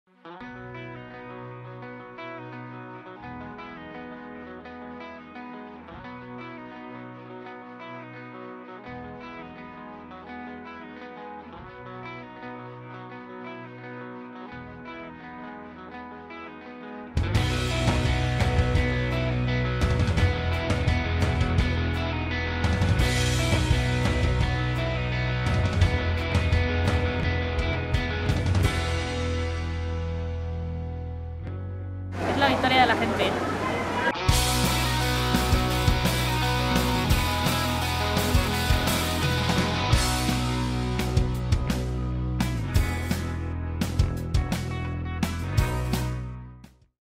Presentació RutaAlaFresca cap al 26J
La RutaAlaFresca és una forma distinta de fer campanya: carregats amb candidates, materials, orxata, i ganes de canvi, Podemos recorre la Comunitat Valenciana des del sud fins al Nord, des de la Vega Baja fins a Vinaròs, per a assolir la victòria de la gent el pròxim 26 de juny i guanyar un país que no deixe a ningú enrere.